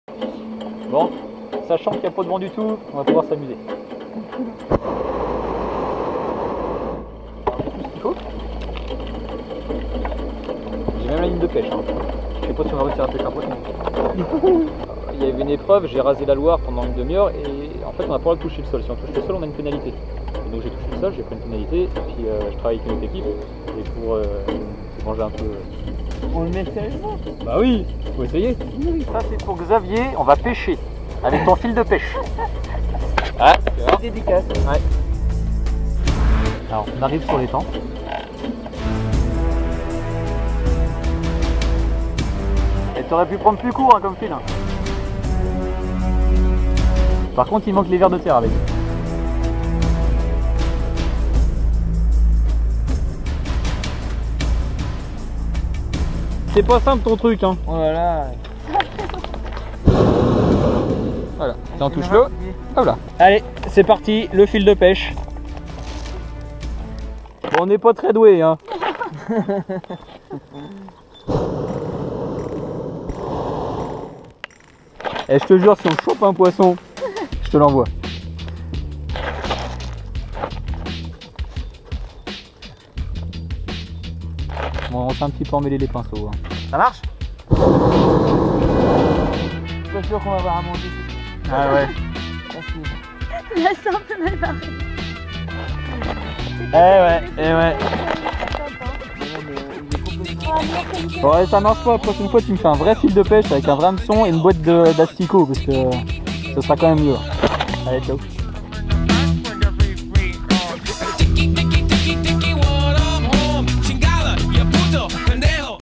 Pêche amusante, insolite en montgolfière défi Team Vendée avec la team OBO et Rocamadour
Au mois de juin nous avons eu l'occasion d'essayer notre ligne de pêche après 45 minutes de vols nous nous sommes posé sur un petit plan d'eau ou nous avons essayé de pêcher sans grand succès histoire de s'amuser